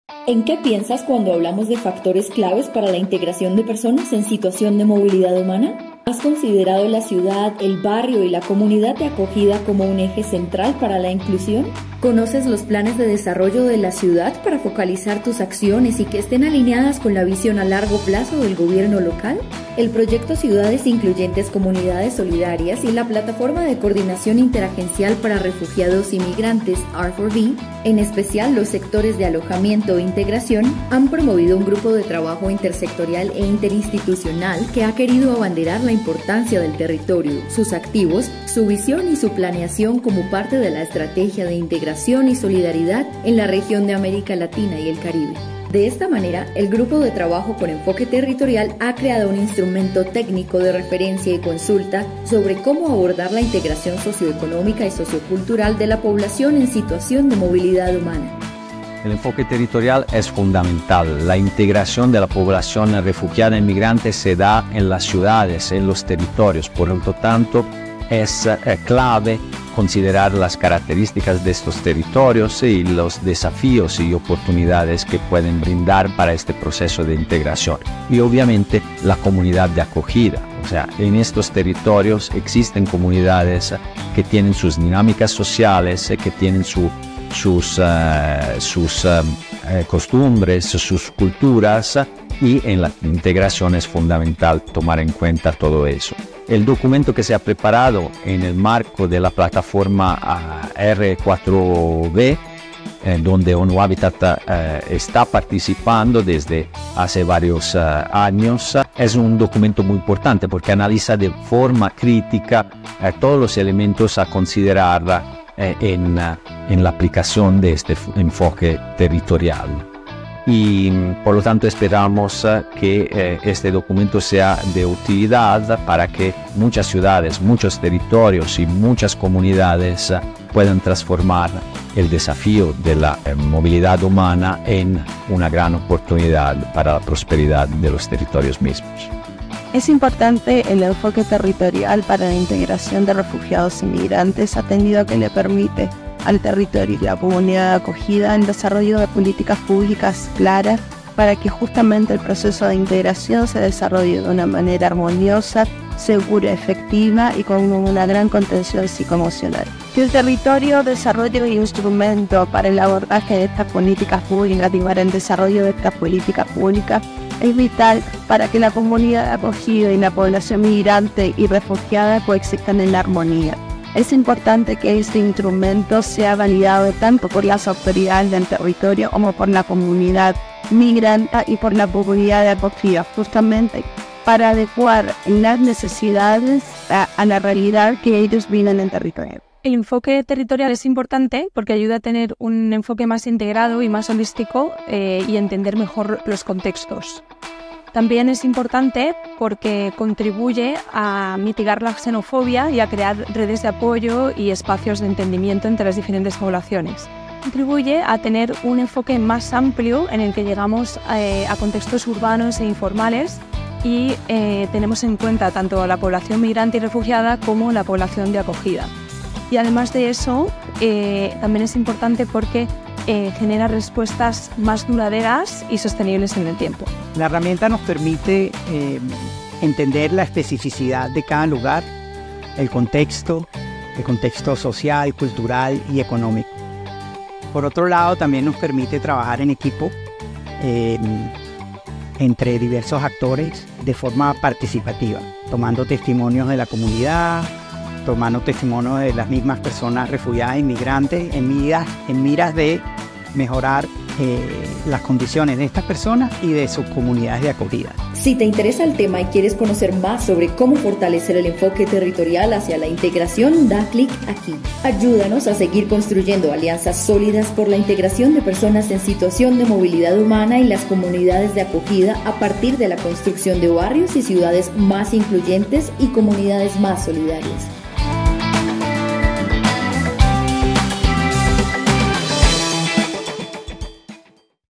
Strengthening the Territorial Approach to Integration
Building on the document 'Strengthening the Territorial Approach to Integration', this video explores the advantages of using a territorial approach to promote the socio-economic and socio-cultural integration of refugees and migrants. By fostering solidarity, collaboration, social cohesion, and peaceful coexistence, this process enables them to become key contributors to prosperity, development, and innovation.